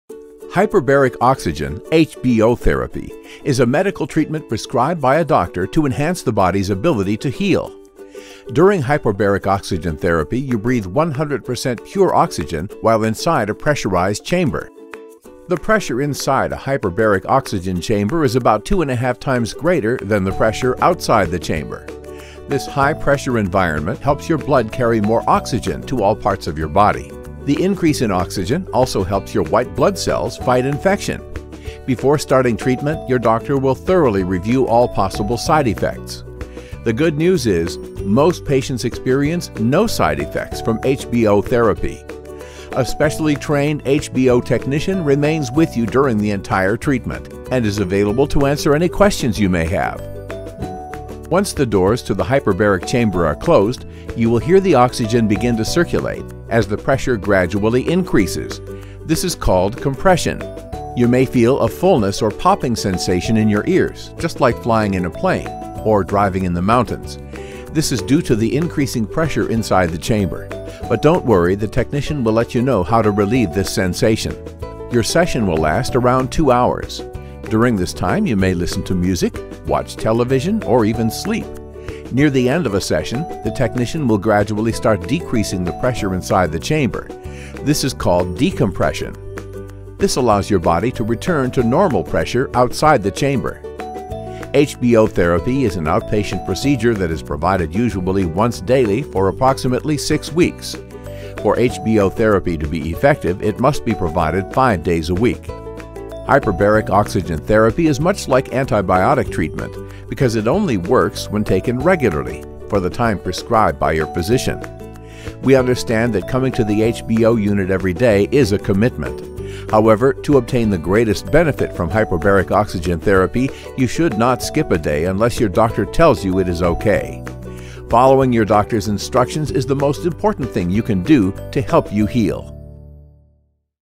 Hyperbaric Oxygen Therapy
Learn about Hyperbaric Oxygen Therapy, an innovative treatment option offered at Morton Hospital's Center for Wound Healing.